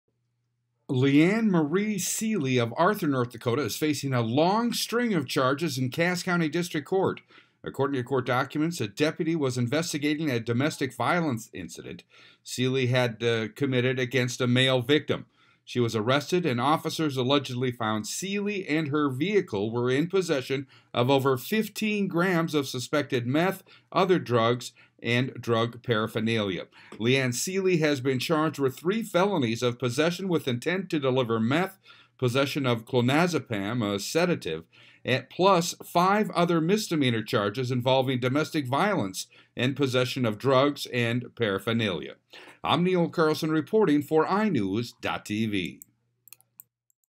Woman Facing Long String Of Charges In Cass County, ND
ARTHUR, ND (iNewZ.TV) Leann Seely of Arthur, North Dakota is facing a long string of charges, after a Deputy investigated an alleged Domestic Violence incident.